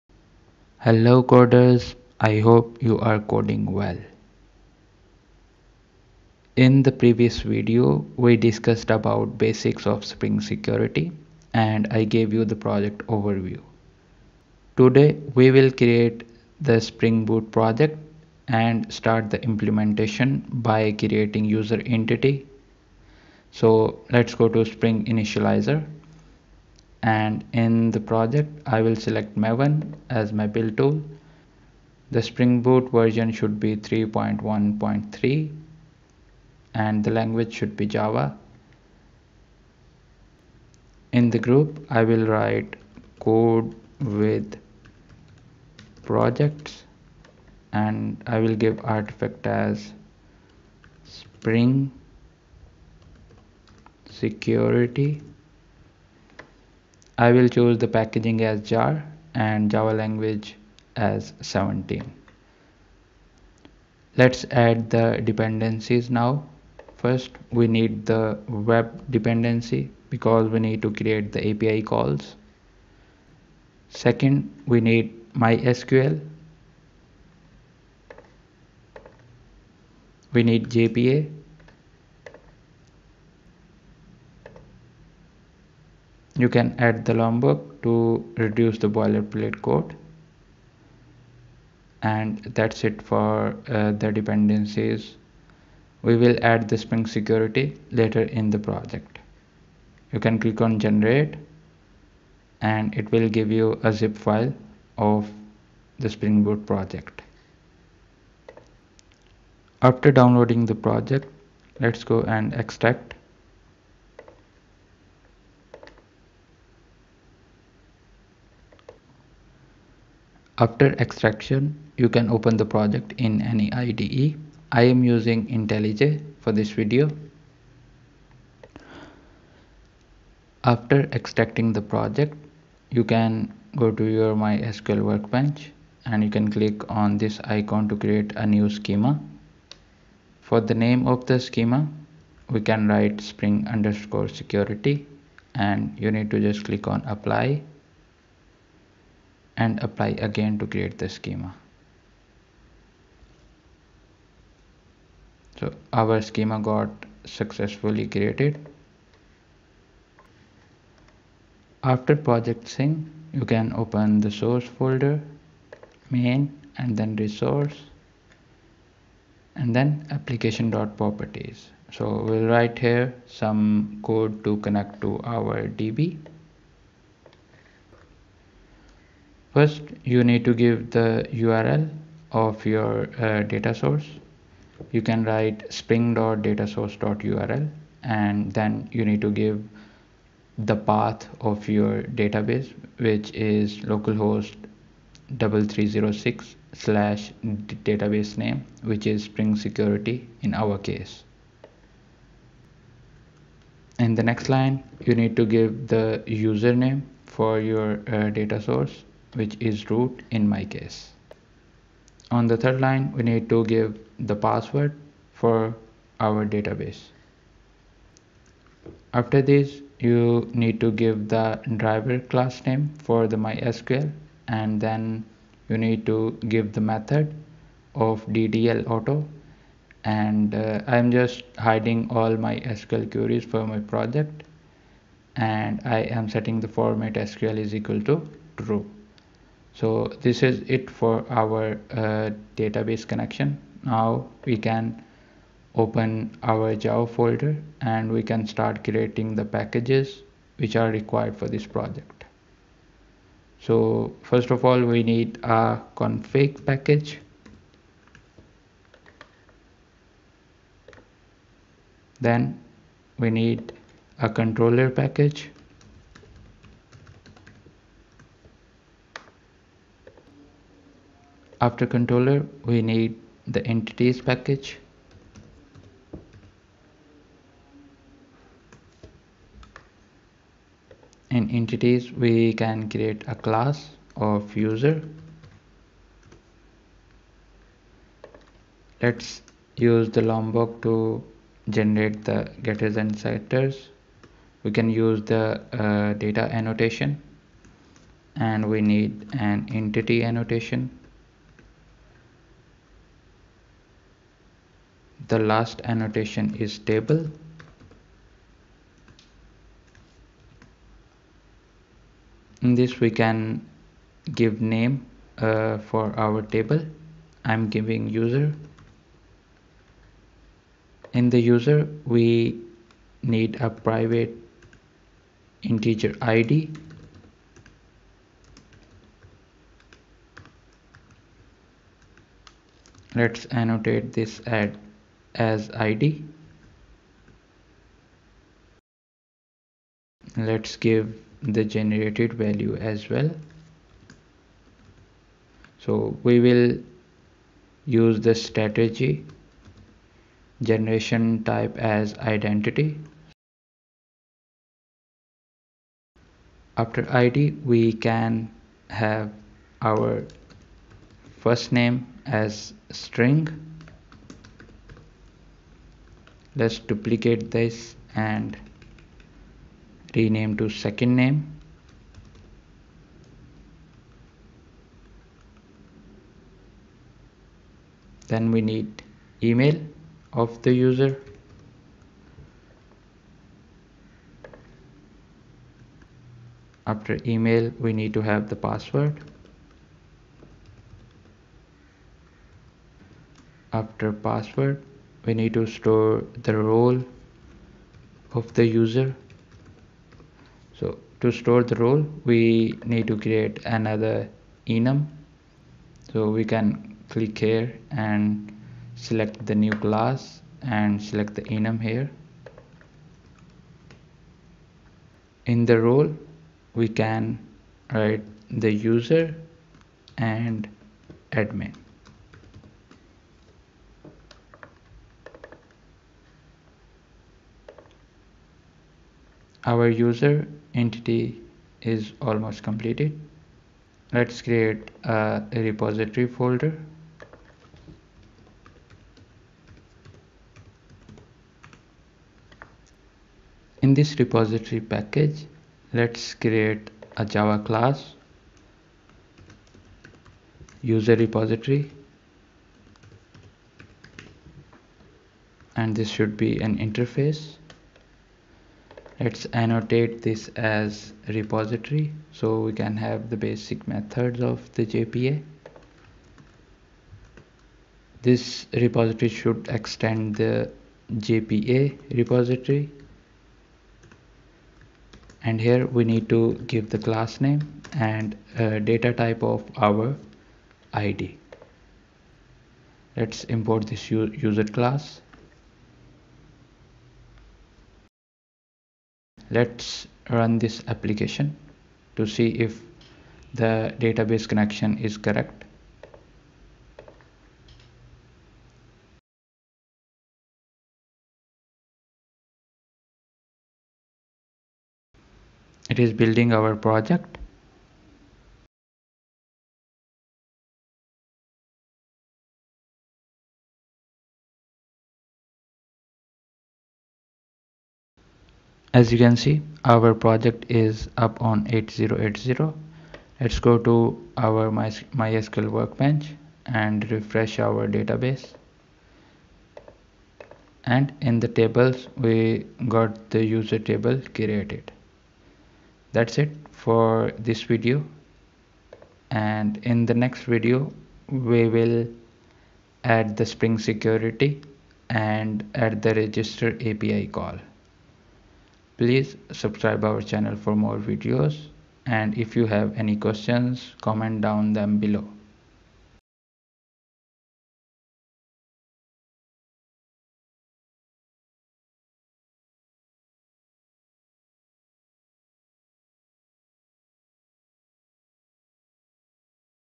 Spring Boot 3.1 With JWT | Project Setup, Database Connection And User Entity | Part 2
In this video, our objectives are as follows:

We will initiate the creation of a new project using Spring Initializer and configure properties for establishing a database connection.
Subsequently, we will proceed to develop a user entity, which will include a role enumeration.
To facilitate data interaction, we will also establish a user repository.